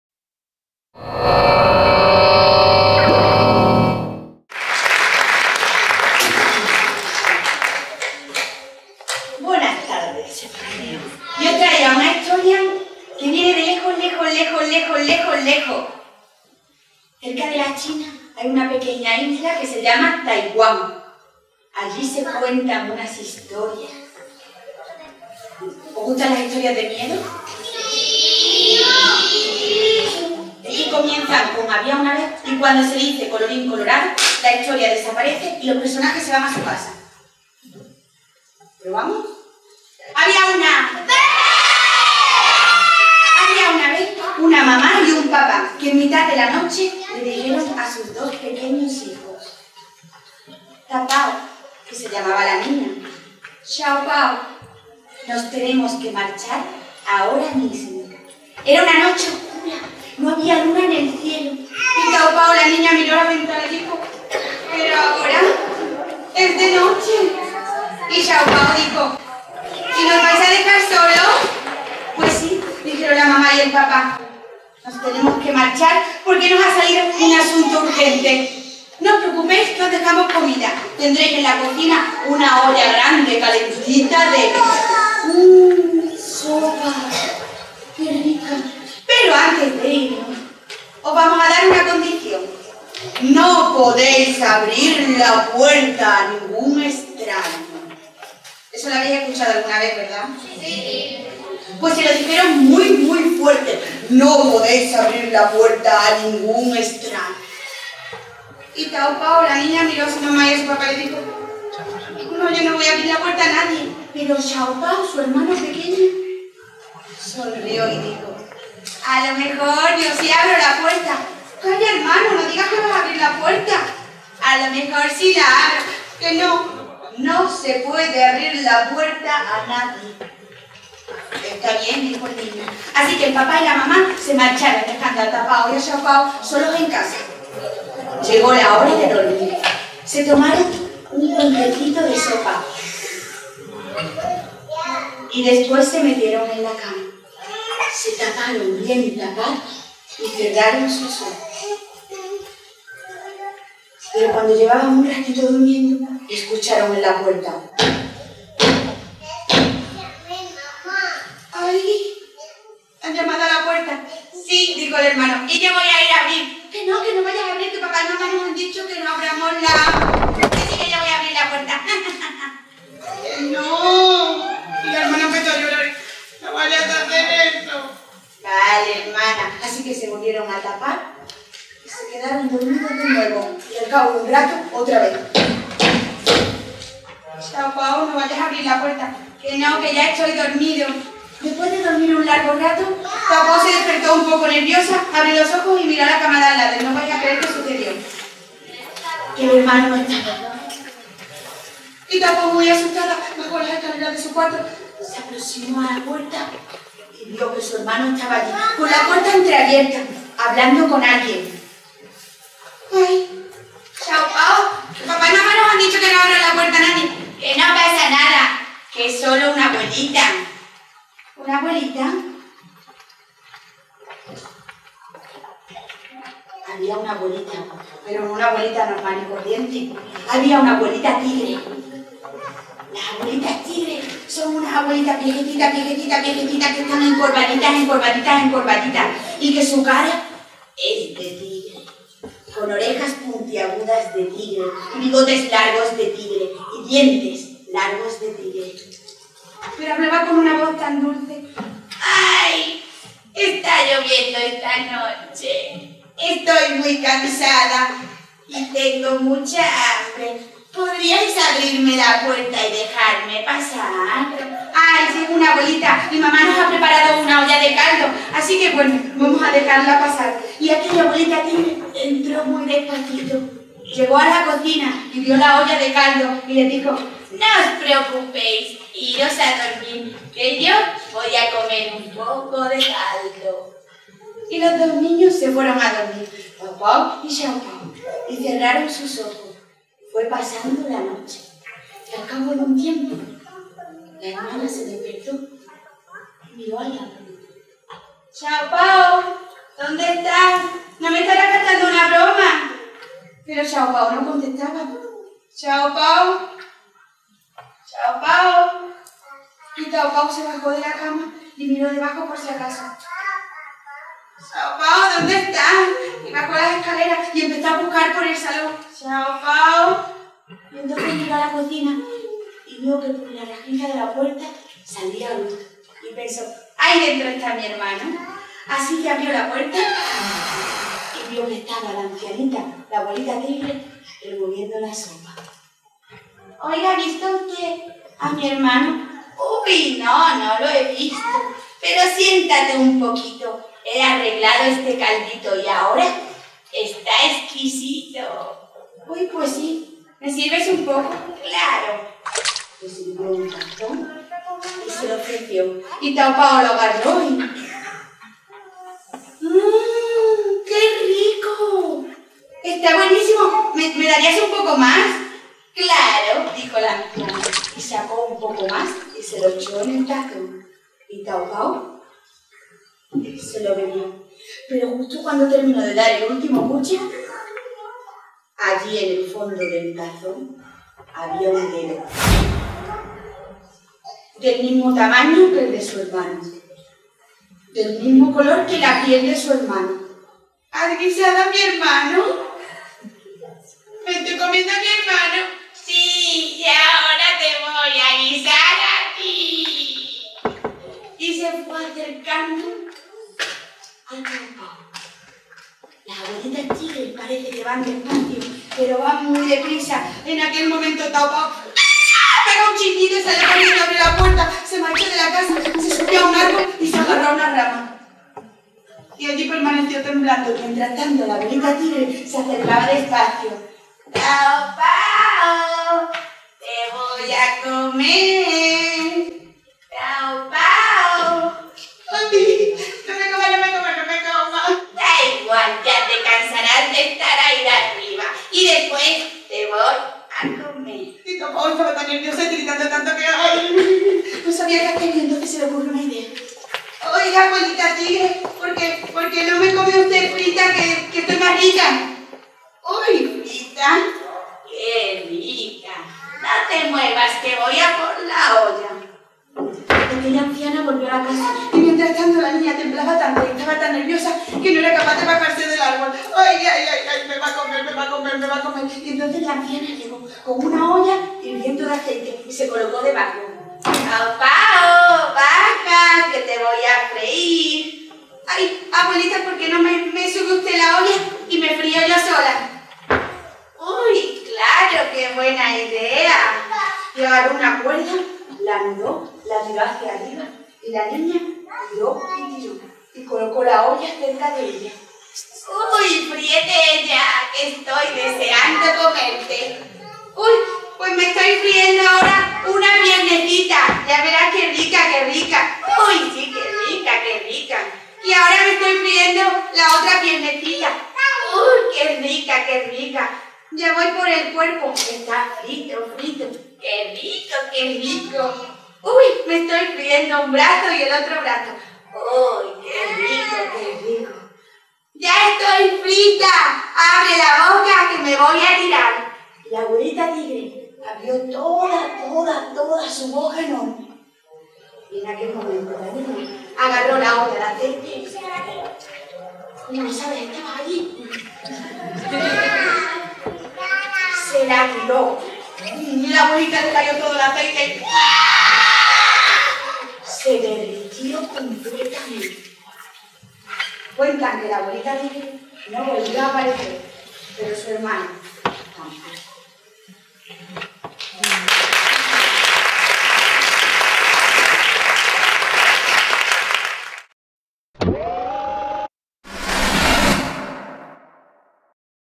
5º Festival Cuentacuentos Rompenubes - Alicia Remesal - La abuela tigre y los niños
Después de escuchar este cuento no te quedarán ganas de abrir la puerta a ningún extraño en ausencia de tus padres.

Es originario de Taiwán de los años 60. Se conocen 241 transcripciones y tiene ciertas similitudes con los siete cabritillos de los hnos. Grimm. y Caperucita Roja de Perrault. El lobo en este caso es sustituido por un tigre.